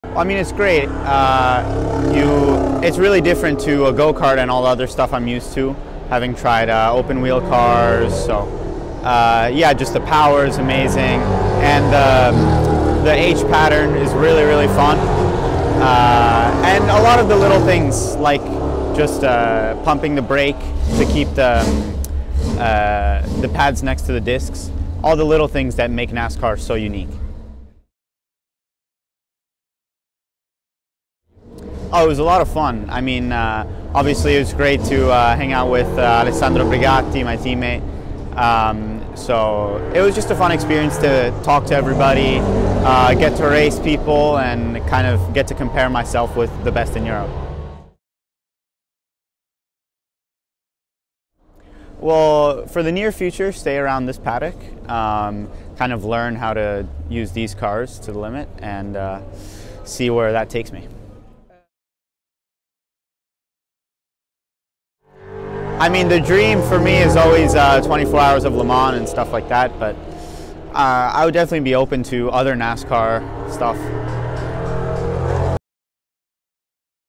Leonardo Colavita ready to make his EuroNASCAR debut
16-year-old Italian driver Leonardo Colavita is ready to take on EuroNASCAR in both championships plus the Club Challenge, this weekend at Grobnik. Here's what he thinks about driving a EuroNASCAR car and what he expects from this first season.